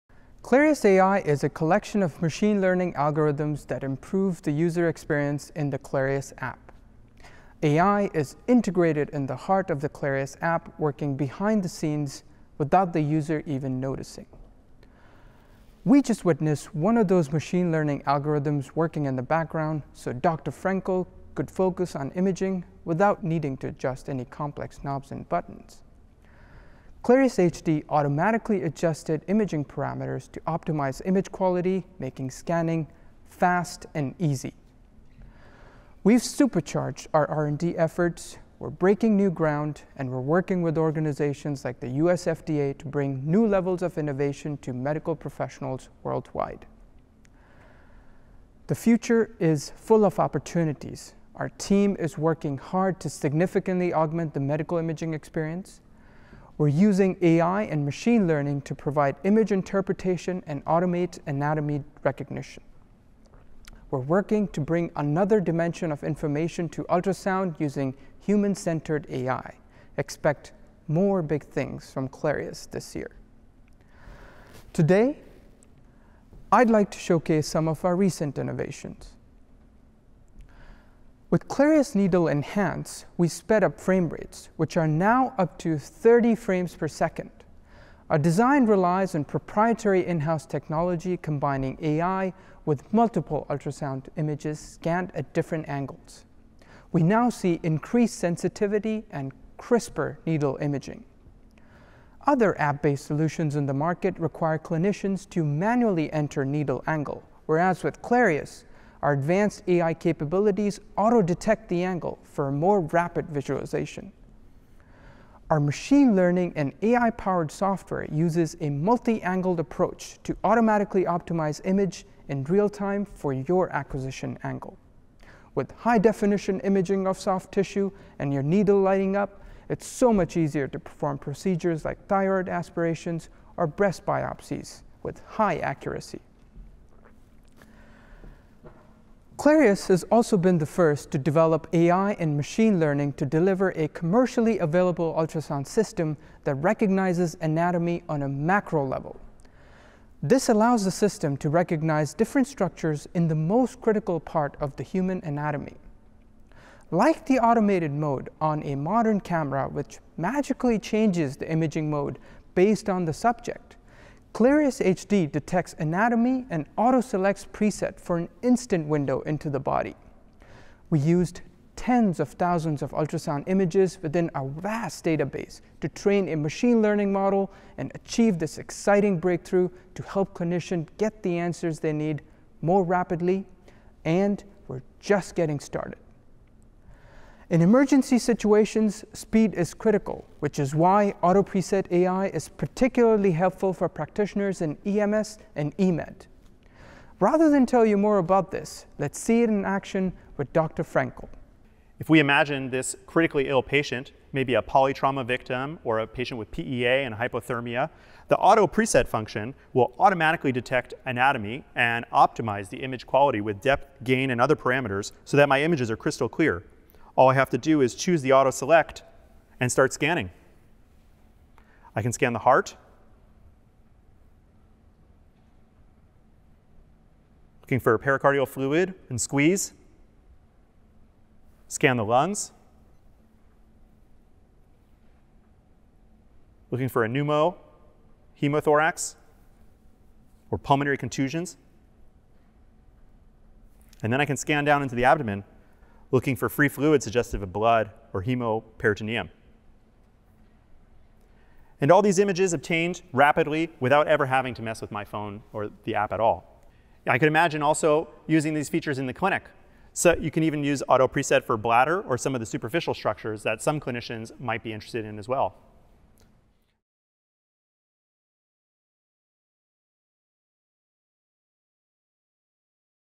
Innovation Spotlight: How is Artificial Imaging Used with Clarius Ultrasound?
Clarius Artificial Imaging (AI) is a collection of machine learning algorithms that improves user experience in the Clarius app. AI is integrated at the core of the Clarius app and constantly works behind the scenes.

With the release of the 3rd generation Clarius ultrasound, we’ve supercharged our research and development efforts. We’re breaking new ground with AI every day and we’re working with organizations like the US FDA to bring new levels of innovation to medical professionals worldwide.

To see how AI and machine learning are enhancing image quality and automating image acquisition, we invite you to watch a brief video clip featuring Nishant Uniyal, our Director of Data Science, which we recorded at our recent Innovation Spotlight.